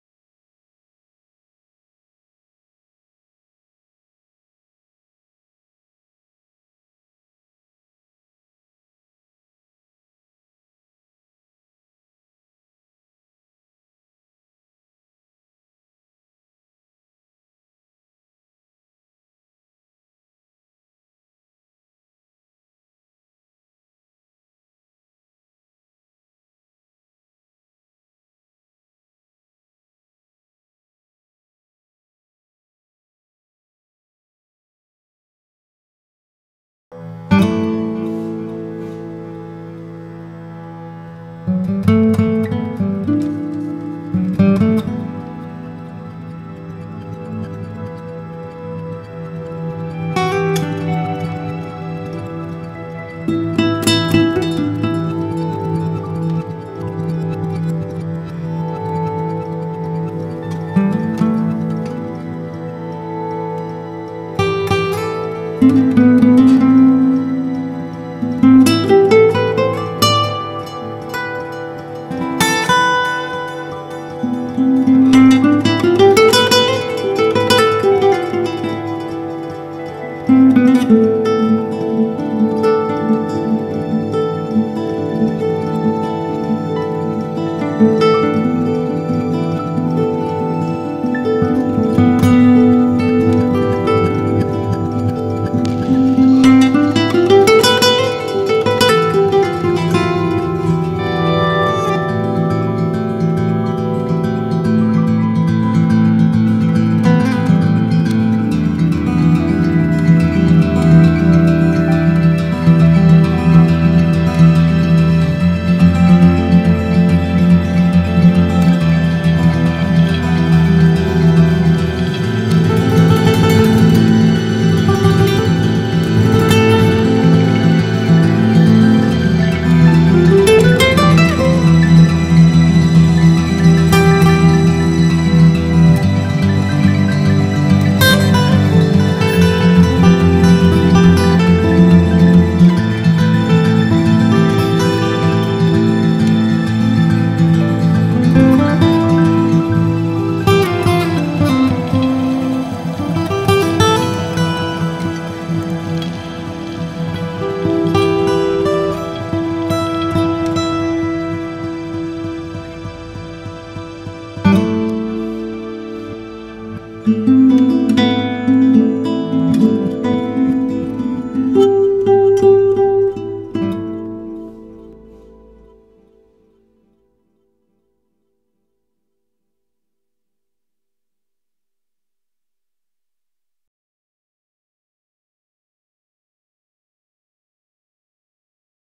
Quinta do Noval : le film
Un film saisissant qui plonge d'emblée au cœur de l'univers de Quinta do Noval : ses paysages à couper le souffle, son terroir unique, la magie de ses vendanges, l'élaboration de ses grands vins... Visitez notre site web

Quinta do Noval est l'une des maisons historiques de portos majeures, reconnue pour la grandeur de ses portos Vintage, pour la finesse de ses vieux tawnies et depuis 2006 pour ses vins du Douro.